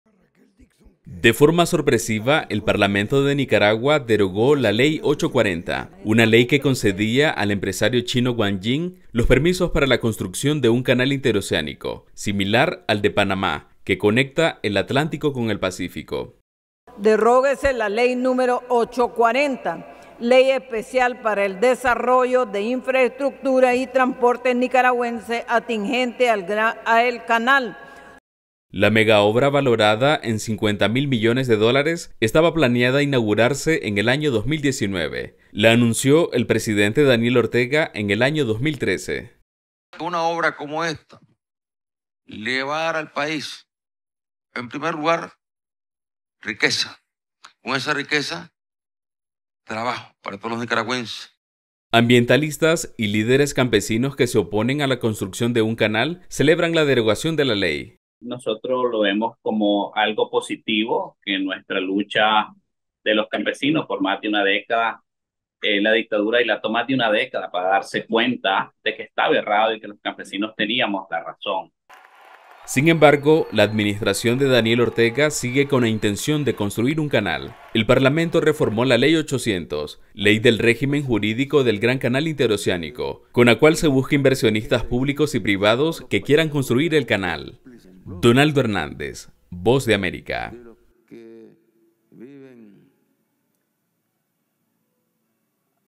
Nicaragua cancela la construcción de canal interoceánico
Nicaragua canceló la construcción de un canal interoceánico que pretendía competir con el panameño. El Parlamento derogó la ley que le cedía a un empresario chino la construcción de la mega obra. Ambientalistas y campesinos celebran esta decisión. Donaldo Hernández, de la Voz de América, nos informa.